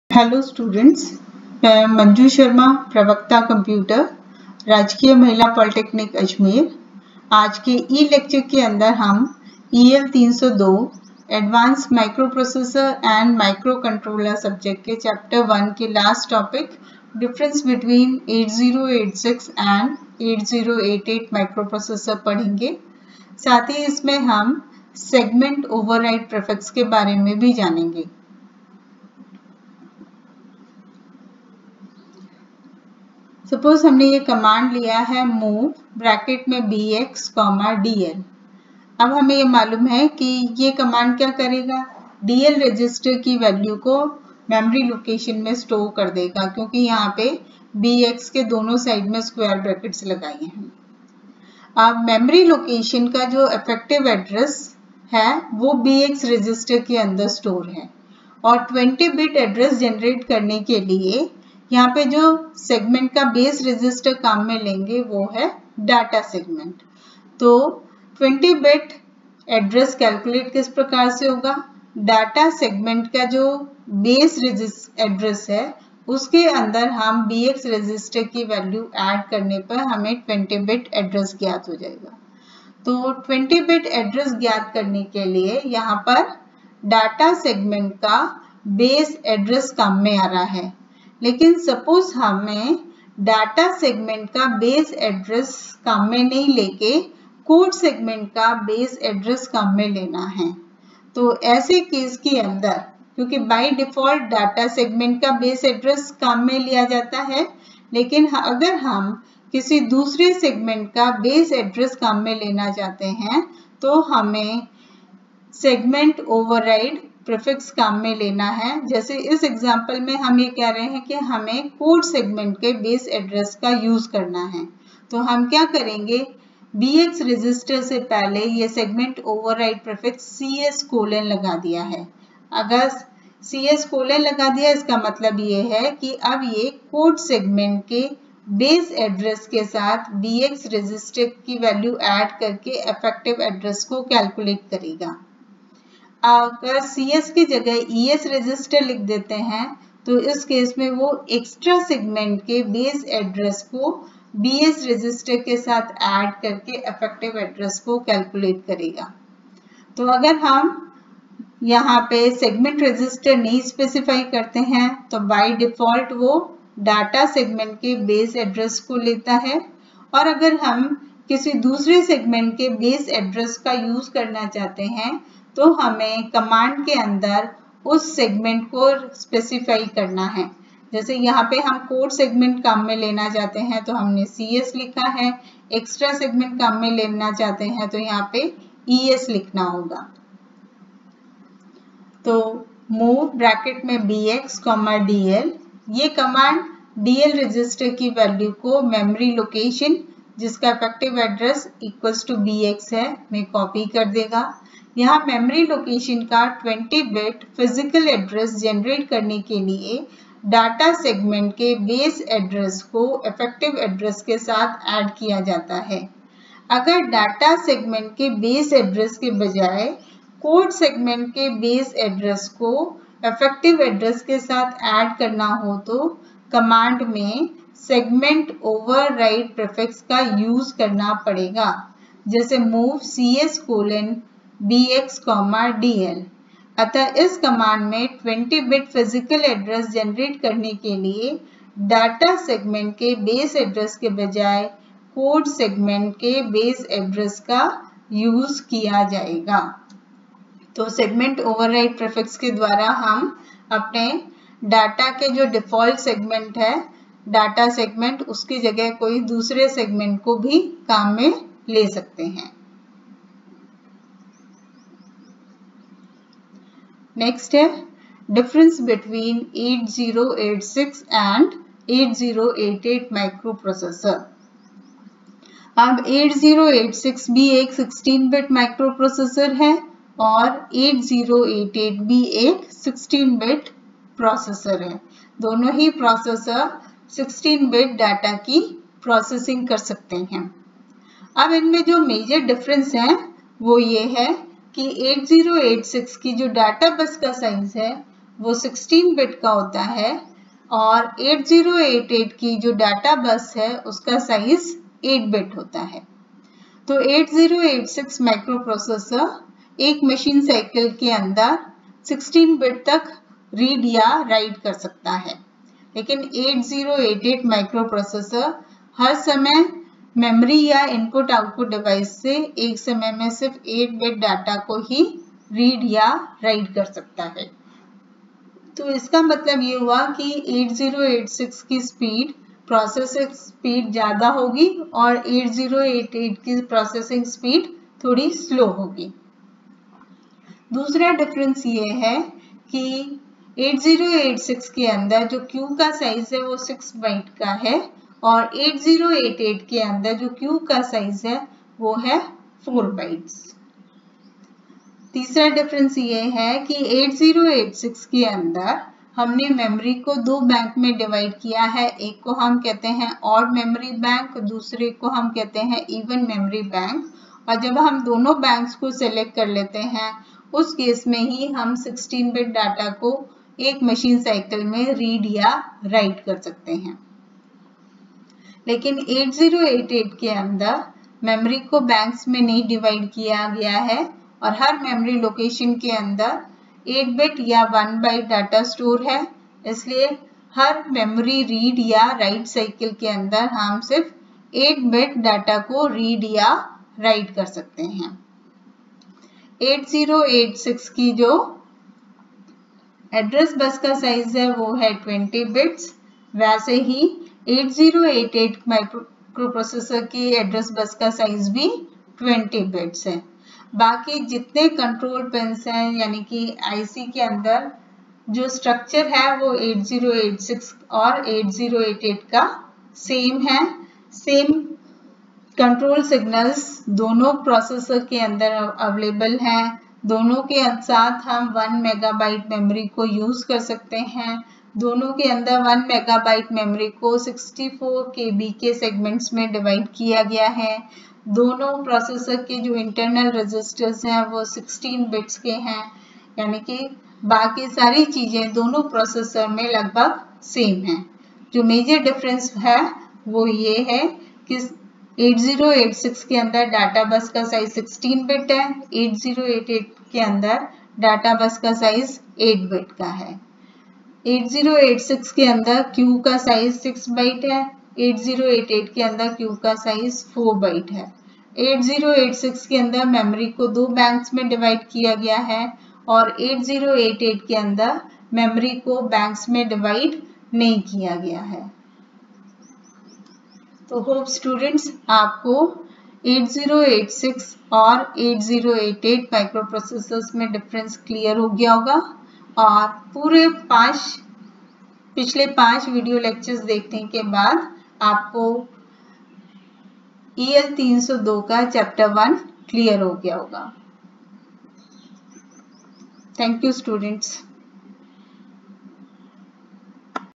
EL302 Comparison between 8086 & 8088 by Manju Sharma Lect. Computer GWPC ajmer
EL302 Topic 1.5 Comparison between 8086 & 8088 as per BTER syllabus by Manju Sharma Lect. Computer GWPC ajmer